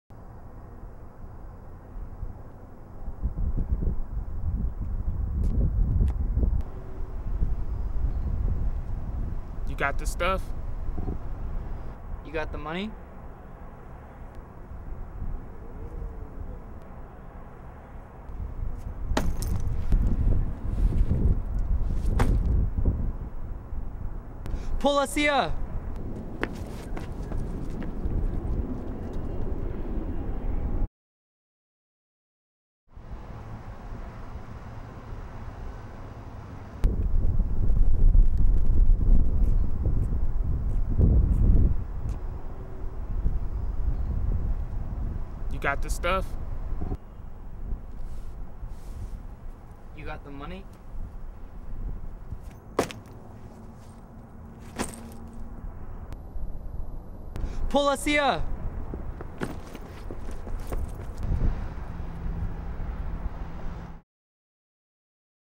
The Drug Deal (Ver. 1 & 2)
This is a short video i shot for an assignment for my editing class. we had to shoot a scene where we follow the "180 degree rule" and than we had to shoot it where we break the "180 Degree rule".

So Version 1 is us following the rule and Version 2 is us breaking it.

And if you dont know what the "180 Degree Rule" is, please google it. Because if i were to explain it than i will just mess with your mind. hahah